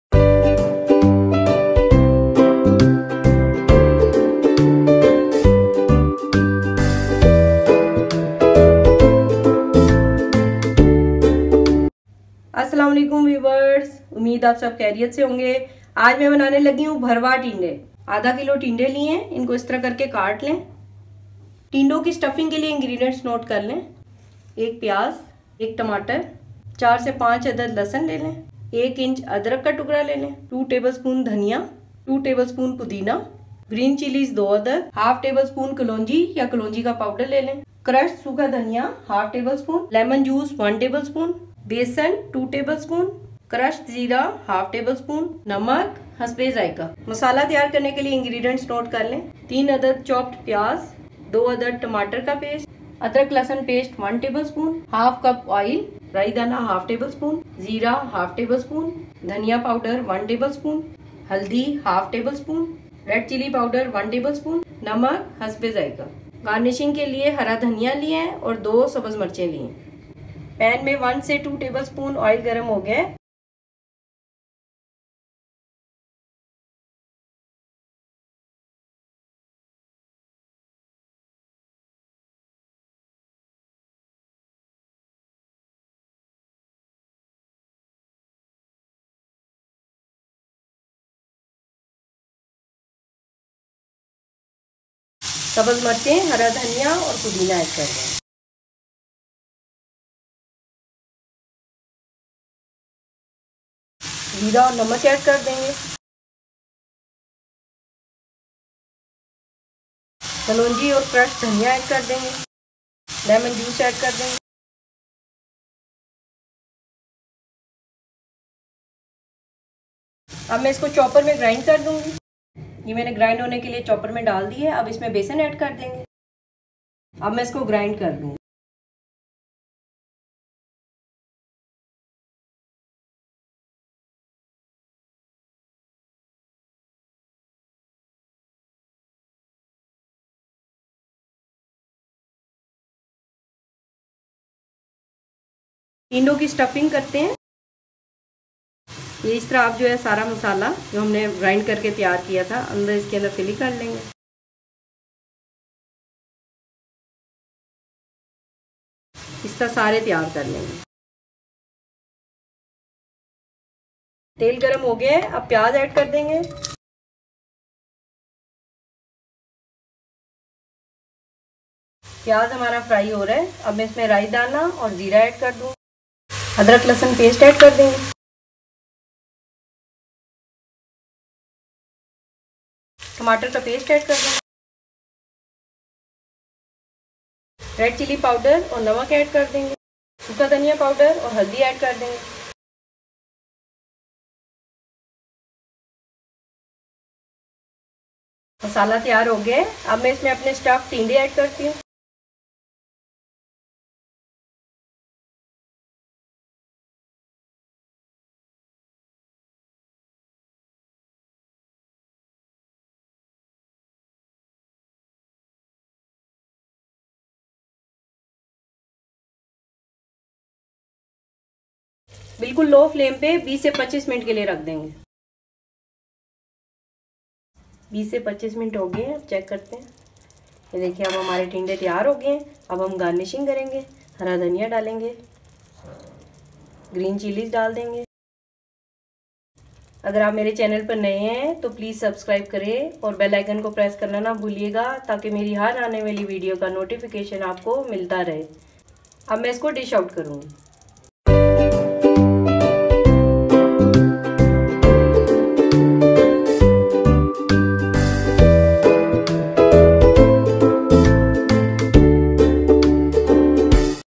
Stuffed Tinday | Bharwan Tinday
——ingredients——

Stuffing ingredients

Apple gourd 1/2 kg
Onion 1
Tomato 1
Garlic 4-5
Mint leaves 2 tsp
Green chillies 2
Black seeds 1/2 tsp
Crushed coriander 1/2 tsp
Lemon juice 1 tsp
Gram flour(besan) 2 tsp
Crushed cumin seeds 1/2 tsp

Masala ingredients

Onion 3(chopped)
Tomatoes 2 (paste)
Rai Dana 1/2 tsp
Salt(to taste)
Green coriander & green chillies 2 for garnish